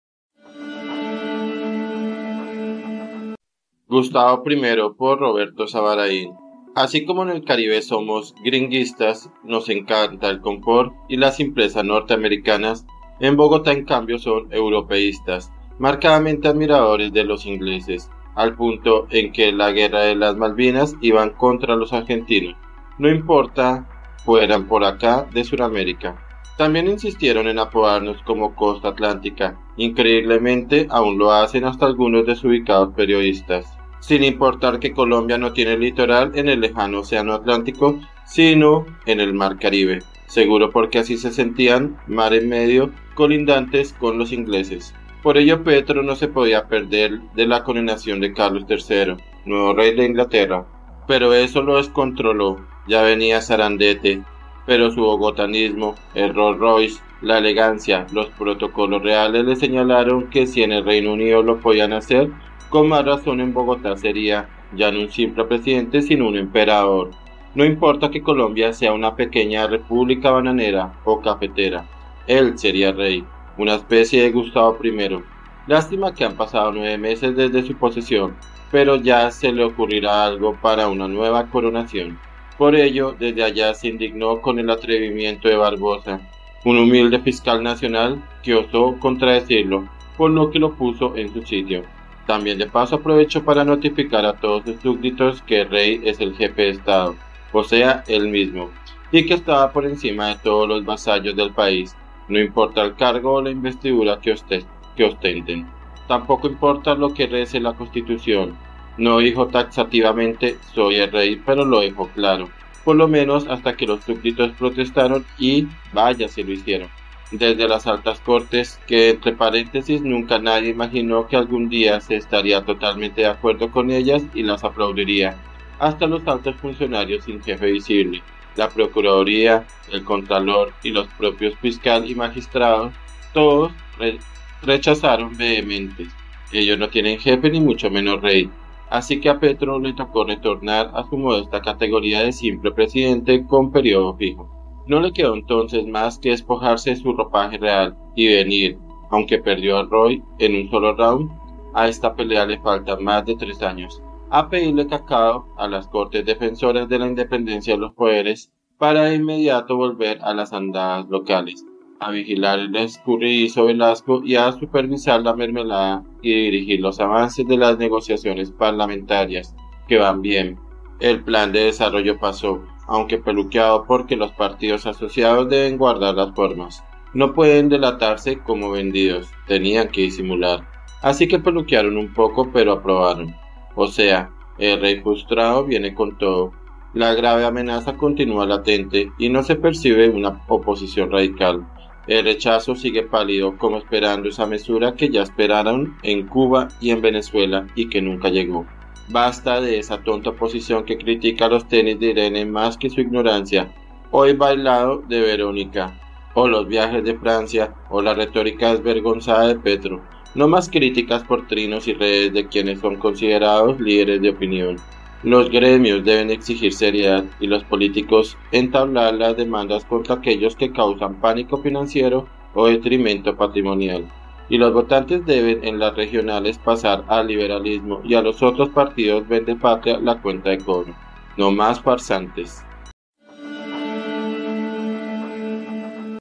GUSTAVO PRIMERO 🔴 Por Roberto Zabaraín
Desde las Altas Cortes, que entre paréntesis nunca nadie imaginó que algún día se estaría totalmente de acuerdo con ellas y las aplaudiría, hasta los altos funcionarios sin jefe visible, la Procuradora, el Contralor, y los propios fiscal y Magistrados, todos, rechazaron vehementes, ellos no tienen jefe ni mucho menos rey, así que a Petro le tocó retornar a su modesta categoría de simple presidente con período fijo.
columna Roberto Zabaraín